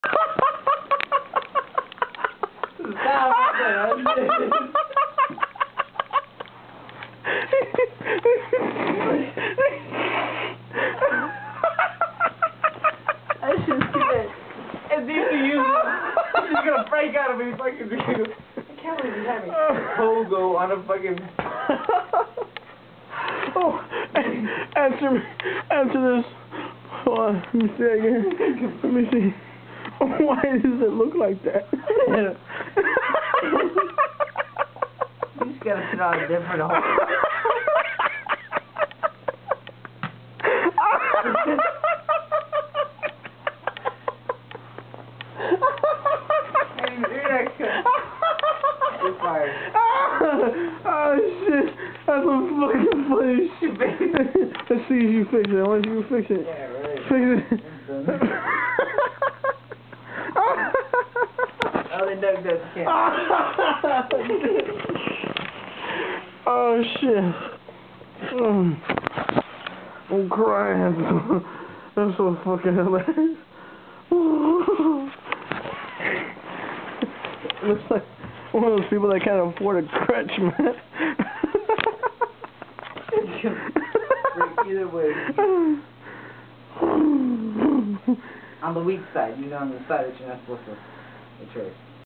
Pogo on 1 crutch Par t2
You put the heel of your shoes in the Y area of a crutch and you see how many times you can bounce on it, but watch out..